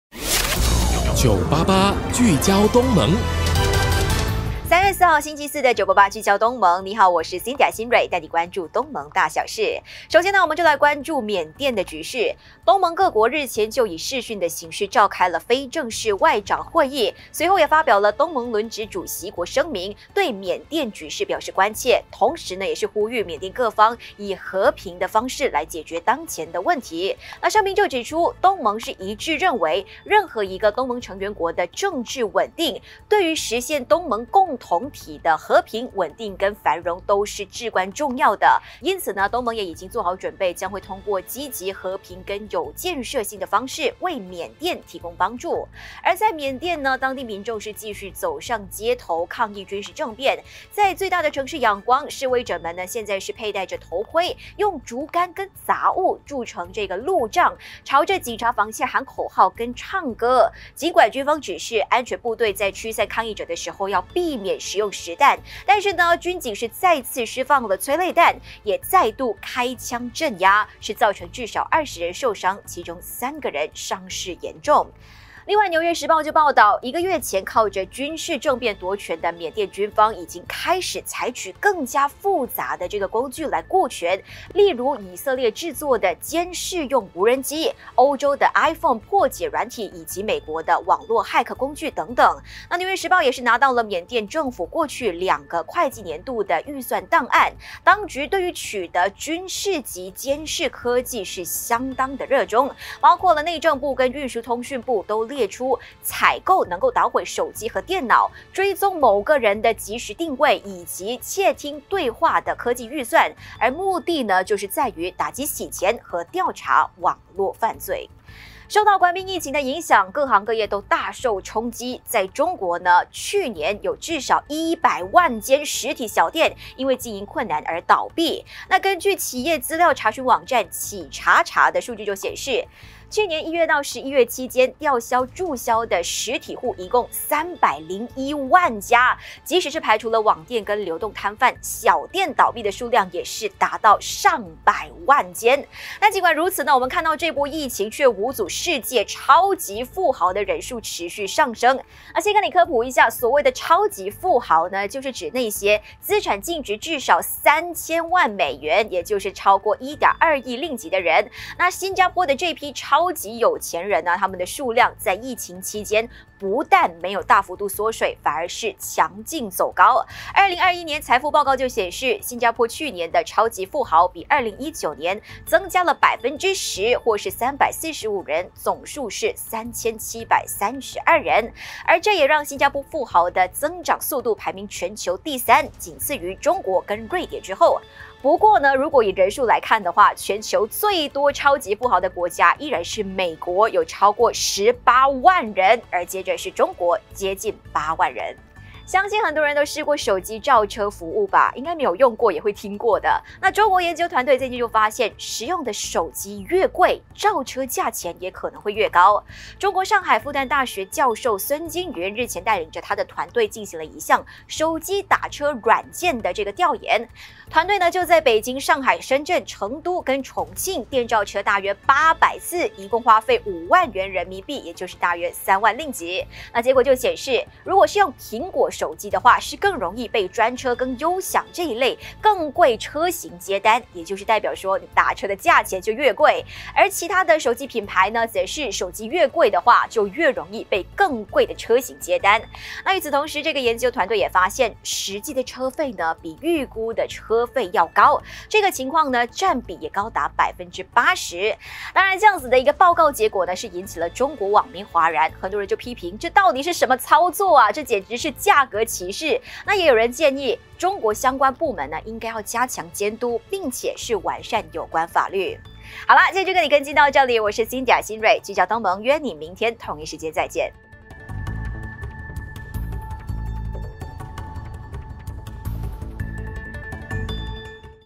【聚焦东盟 04-03-21】中国大学教授调研 越贵手机打车越贵
聚焦东盟: 中国大学教授调研 越贵手机打车越贵

星期一至五，早上9.30am，988聚焦东盟我们LIVE你看！

主播：Cynthia馨蕊